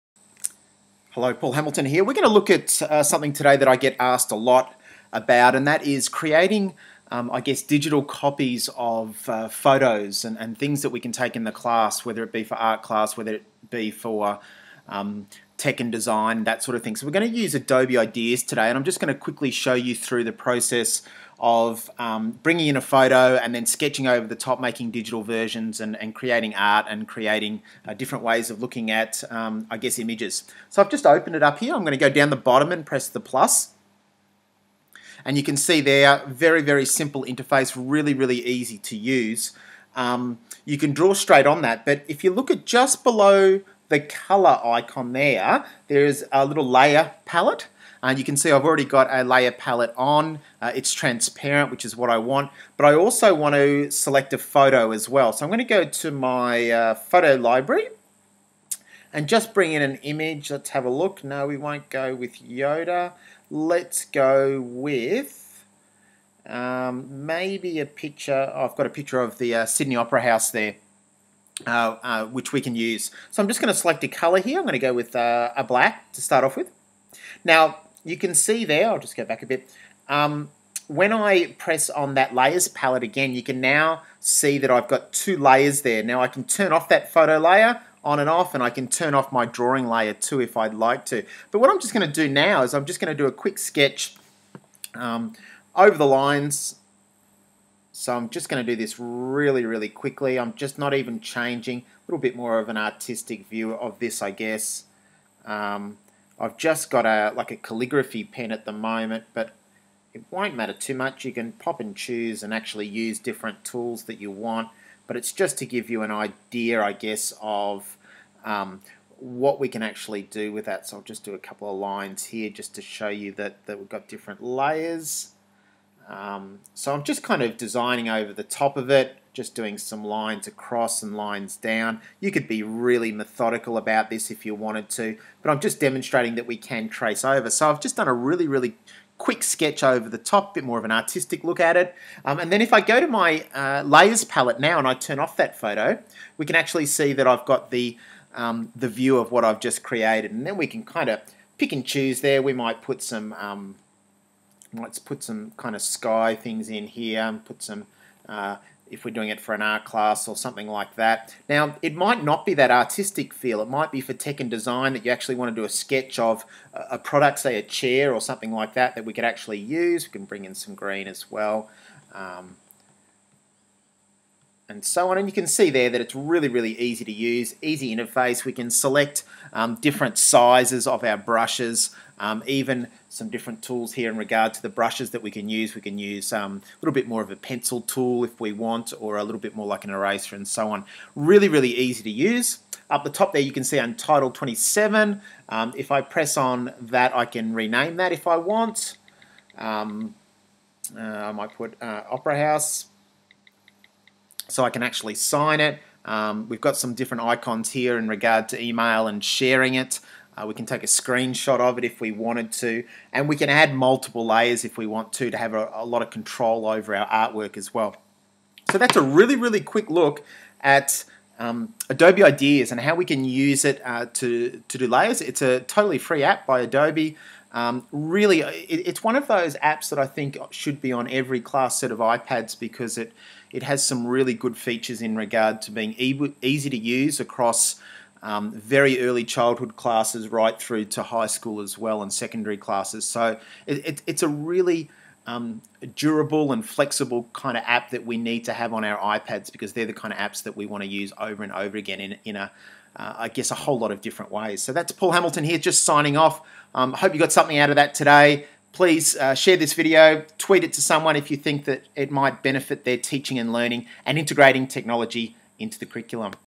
Adobe ideas app
Using Adobe ideas app to create digital copies of images from the camera roll.